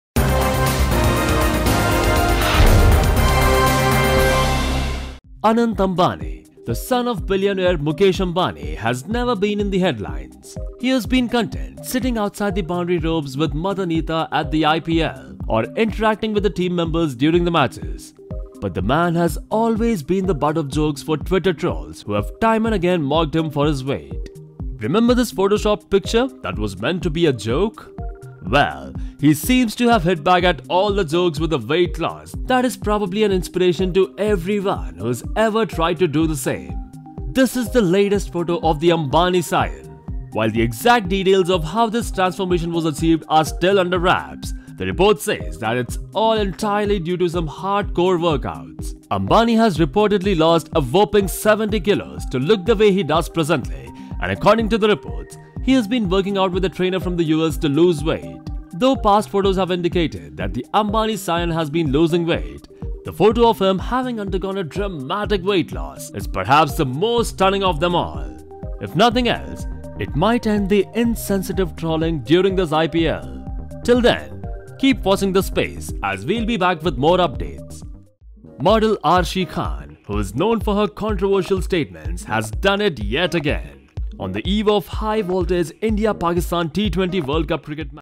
Mukesh Ambani Son Anant Ambani Lost An Incredible 70 Kilos | Mango News
Anant Ambani, Son of billionaire Mukesh Ambani has reportedly lost 70 kgs. Junior Ambani lost this incredible amount of weight which is almost half of what he used to be. According to reports, junior Ambani had underwent a lot of training to achieve this feat and has done plenty of marathon runs at the Reliance refinery in Jamnagar. On top of this, he was also under the constant guidance of a trainer from US.